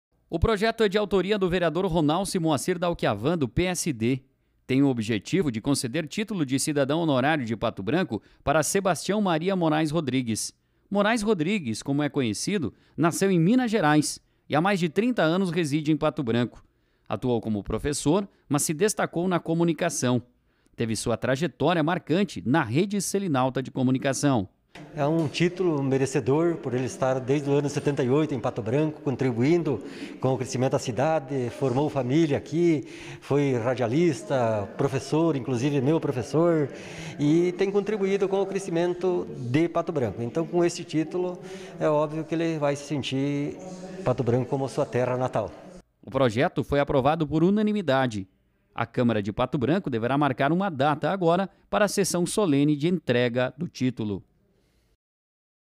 O Professor e Comunicador Moraes Rodrigues irá receber título de cidadão honorário de Pato Branco
Projeto foi aprovado na sessão de segunda-feira (13/07) na Câmara de Vereadores